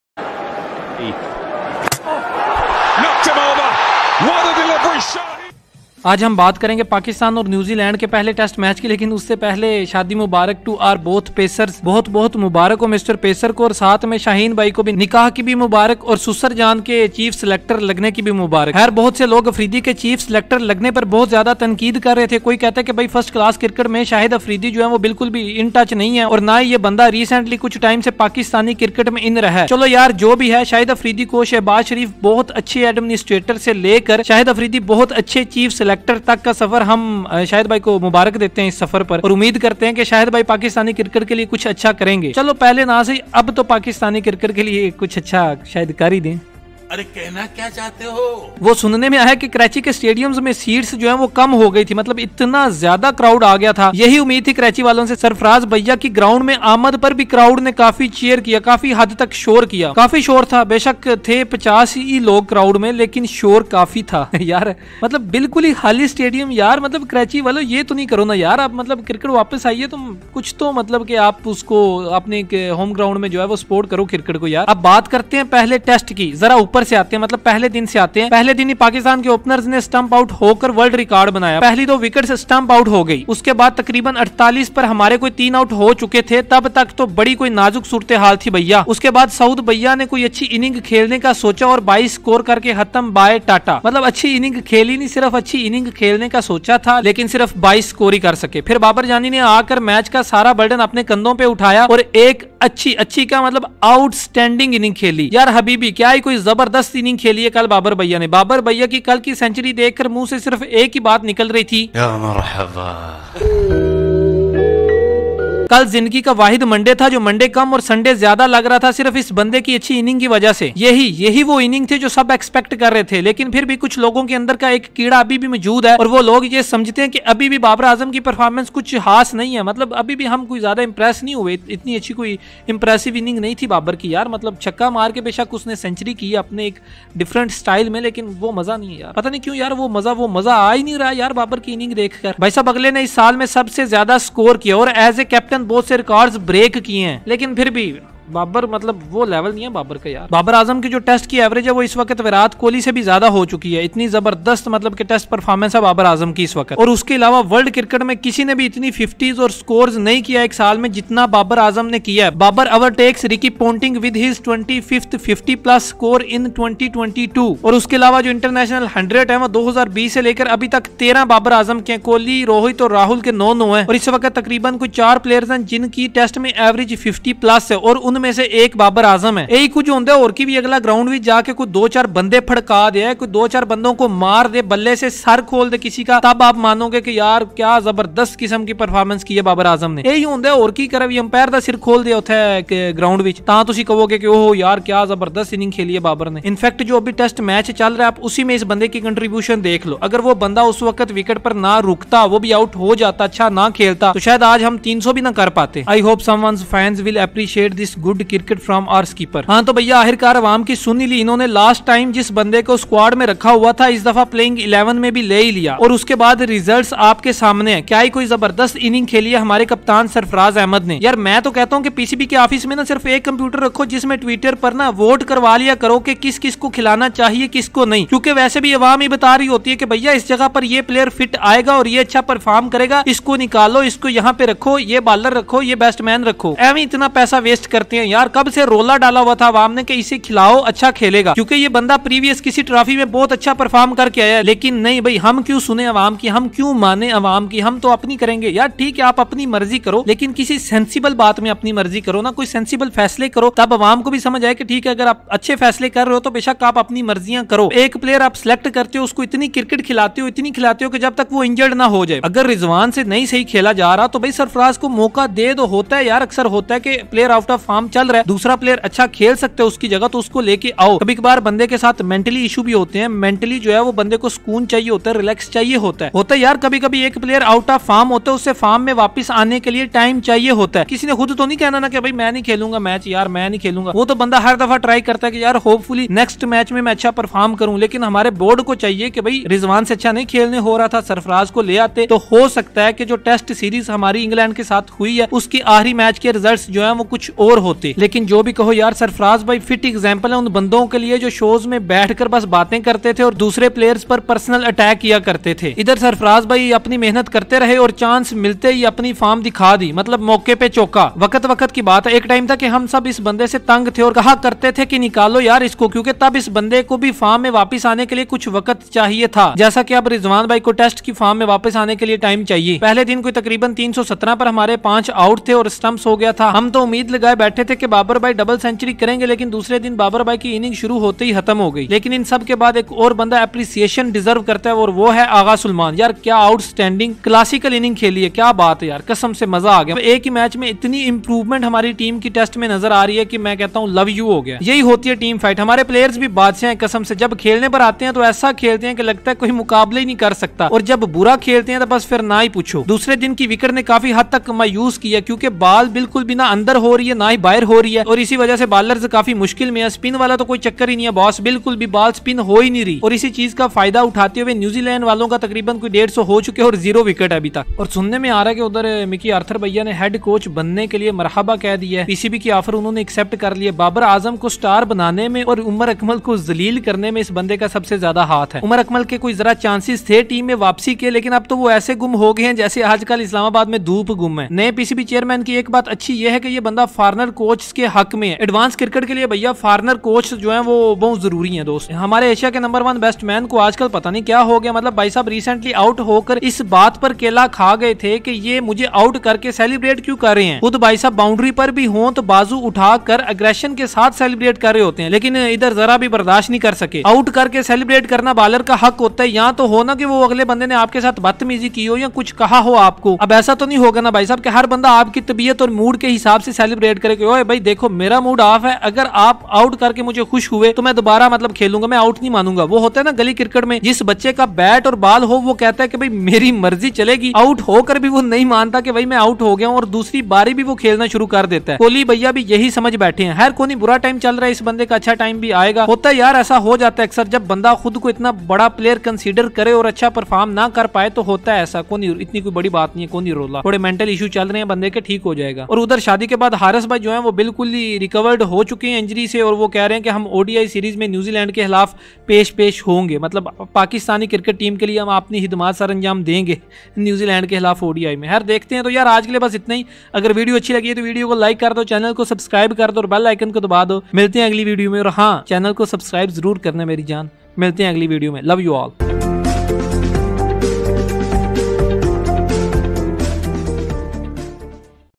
First Test Day 1 & 2 | Pak Vs NZ Series - Babar Azam - Stumps 10
In this Episode of Stumps we'll discuss about the first test of Pak vs NZ day 1,2 and 3.